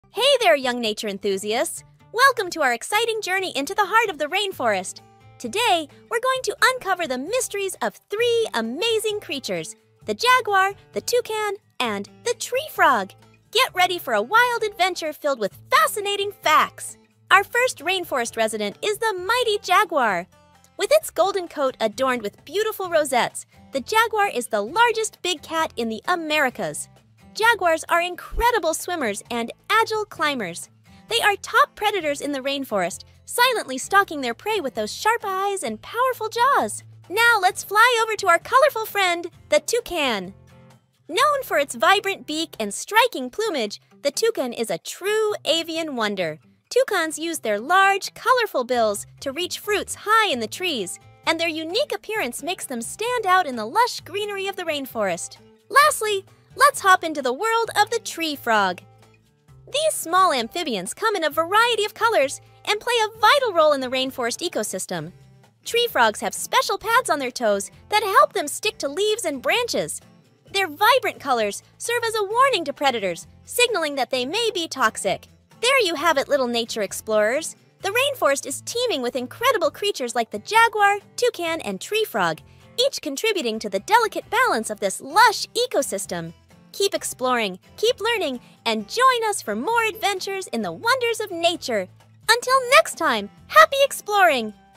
Rainforest Wonders: Meet the Jaguar, Toucan, and Tree Frog! 🌿🐾
Hey little nature enthusiasts! Join us on a thrilling adventure into the heart of the rainforest as we explore three amazing creatures: the mighty Jaguar, the colorful Toucan, and the tiny Tree Frog. Discover their unique features and learn how they contribute to the vibrant rainforest ecosystem. Get ready for a wild ride of fun and education! 🦋🌳